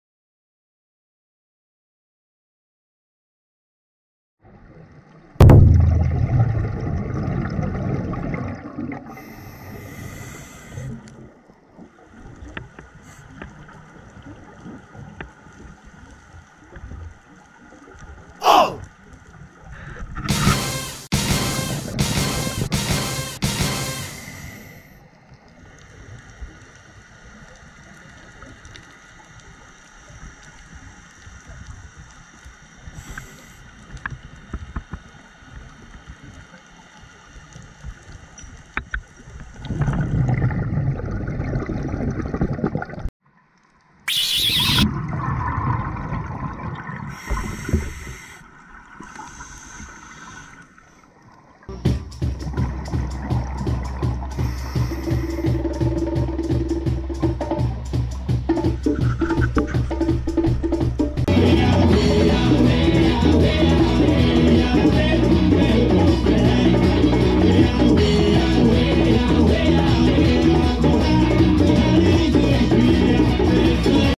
沖縄 ダイビング マイナーポイントの東海岸でダイビング！
沖縄のダイビングは西海岸がメインですが。東海岸でダイビング。
西海岸のダイビングとは違った魅力のある東海岸のダイビング！

沖縄ダイビングショップ・PADIダイビングライセンス